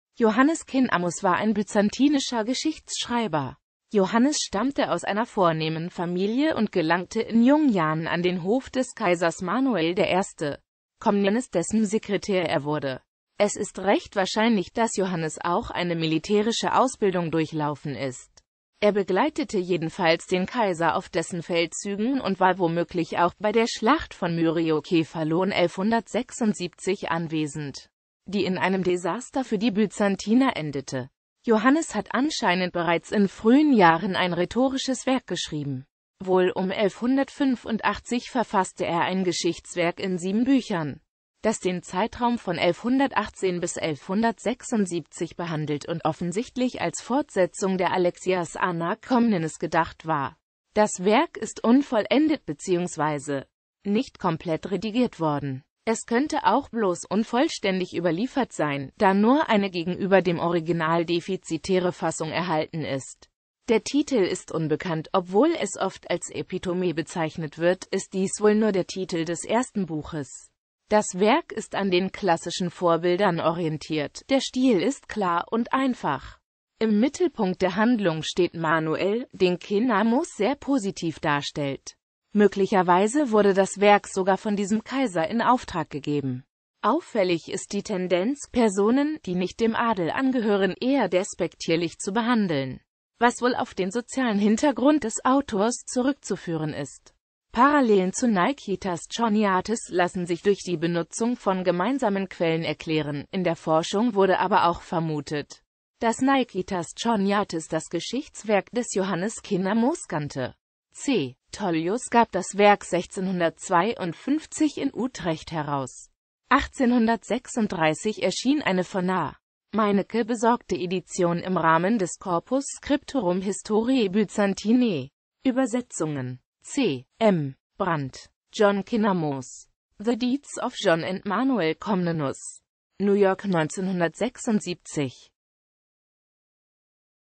Johannes Kinnamos
Vielen Dank für Ihre Unterstützung:

Johannes Kinnamos (mittelgriechisch Ιωάννης Κίνναμος * kurz nach 1143; † nach 1185) war ein byzantinischer Geschichtsschreiber.Johannes stammte aus einer vornehmen Familie und gelangte in jungen Jahren an den Hof des Kaisers Manuel I.Komnenos, dessen Sekretär er wurde.Es ist recht wahrscheinlich, dass Johannes auch eine militärische Ausbildung durchlaufen ist.